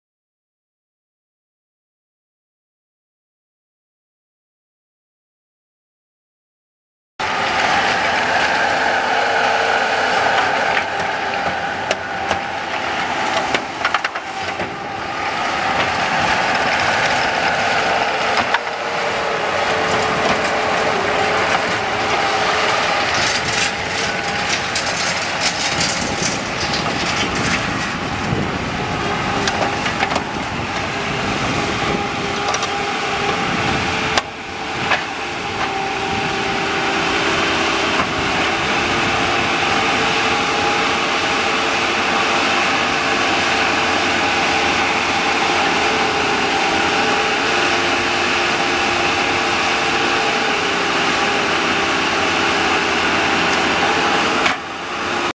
Bobcat T190 track skid steer for sale | sold at auction September 29, 2011
Bobcat T190 track skid steer
2,276 hours on meter
Kubota four cylinder diesel engine
56 HP
Cab with heater
Hand and foot controls
Lap bar and belt
Auxiliary hydraulics
1,900 lb lift capacity
Work lights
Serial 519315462

It was located in Springfield, MO, and we sold it to the highest bidder for $12,650.